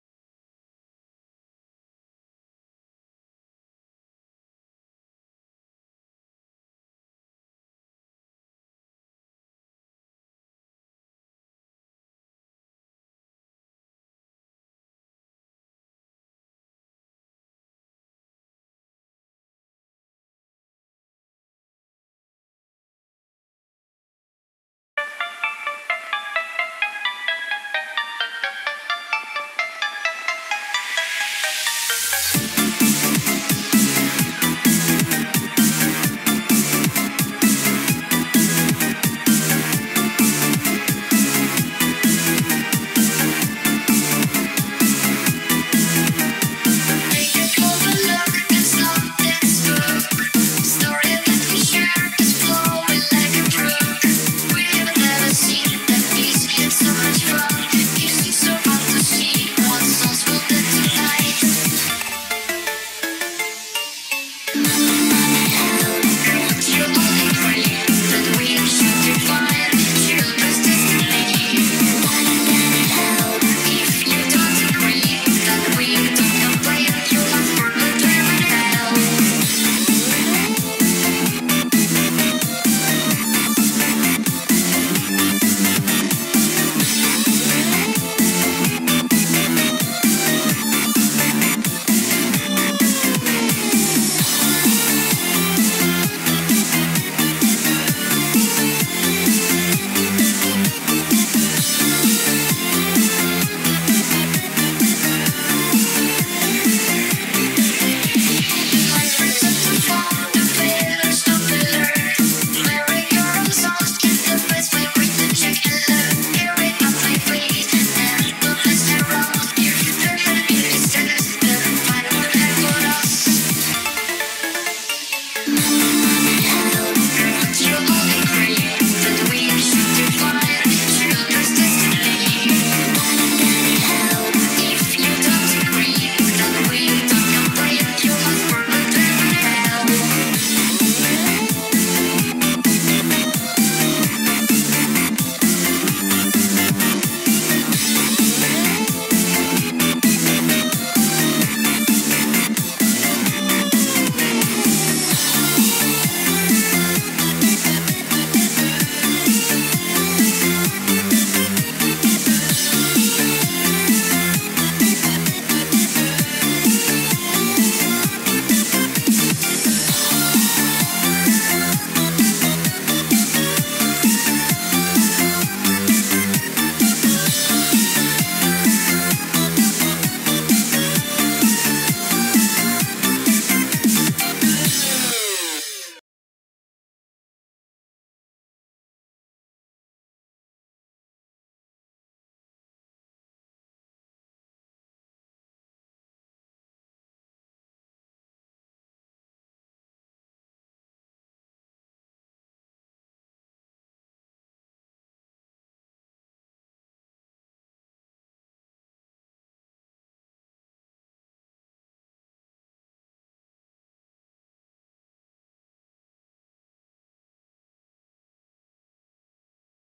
Mha Lyric Prank: "Children's Destiny" 10K Subs Trailer
Note: I know I said the Danplan Video would be next, but I did not expect us to get this close to 10K Subs so quickly, so I apologize, but the next video will be either the Danplan video, or the 10K Subscriber Special, depending on how soon we hit 10K...

Actual Description:

While on a Mission, Kaminari Passes out suddenly, and when he wakes up, William Afton is back from the dead to attack the group!